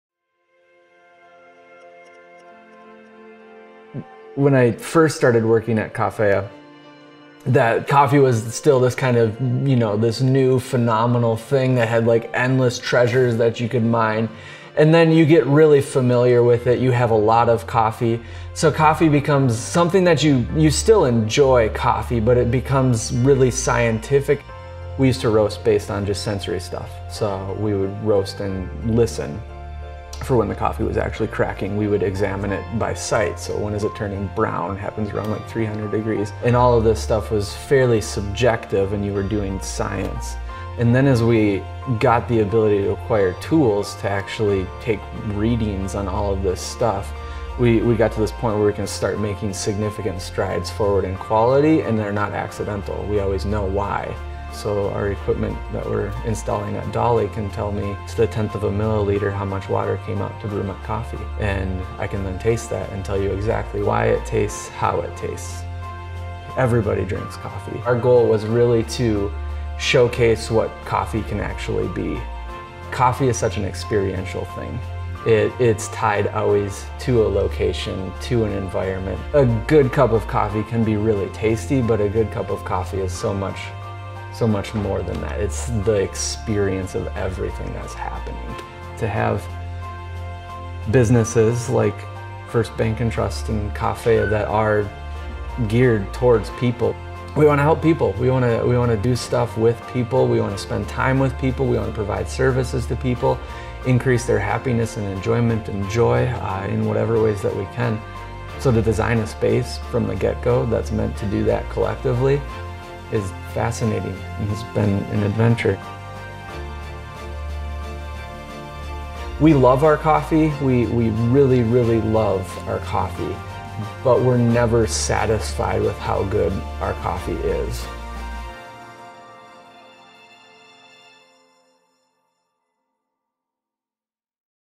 First Bank & Trust - Community with Coffee
There is an incredible human element and experience to coffee...and to banking! Bryan Kegley from Coffea Roasterie explains the art of coffee and the connection that can happen over a good cup.